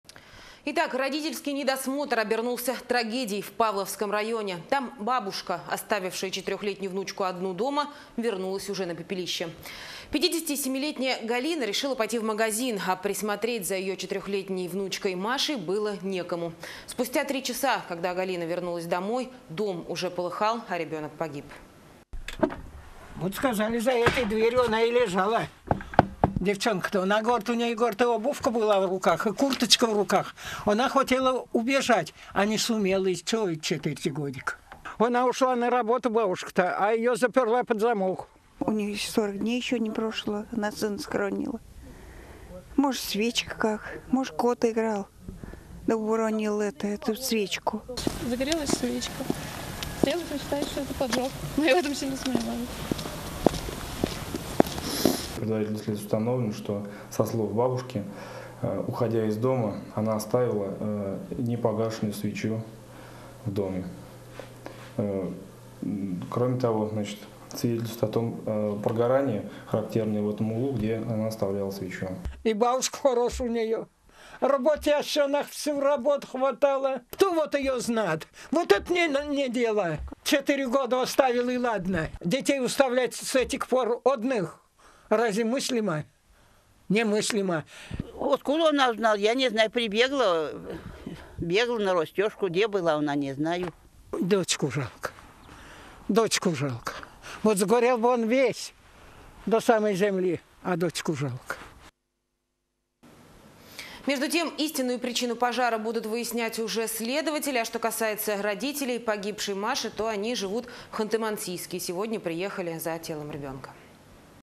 Бабушка, оставившая 4-х летнюю внучку одну дома, вернулась на пепелище
Родительский недосмотр обернулся трагедией в Павловском районе - там бабушка, оставившая 4-х летнюю внучку одну дома - вернулась уже на пепелище. 57 летняя Галина решила пойти в магазин, а присмотреть за 4-х летней Машей было не кому. Спустя три часа, когда Галина вернулась домой - дом уже полыхал, а ребенок погиб. Вот, что рассказывают очевидцы